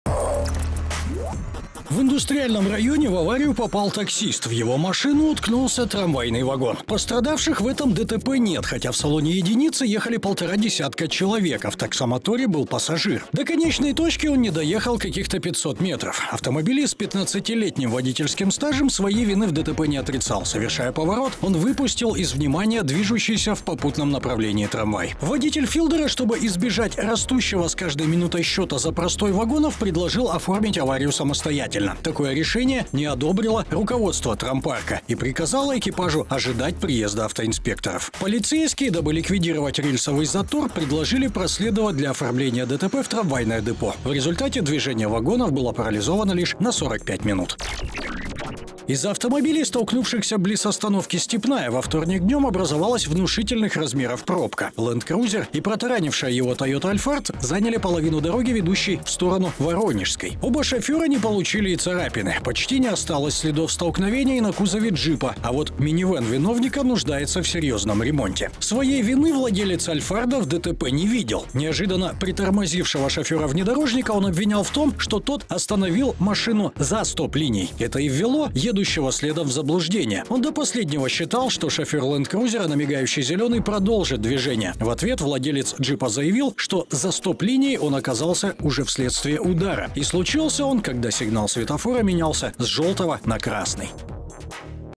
Дайджест аварий в Хабаровске за 16 февраля.MestoproTV
В Индустриальном районе в аварию попали автомобиль такси и вагоновожатый трамвая, сообщает портал «Губерния». Пострадавших в этой аварии нет.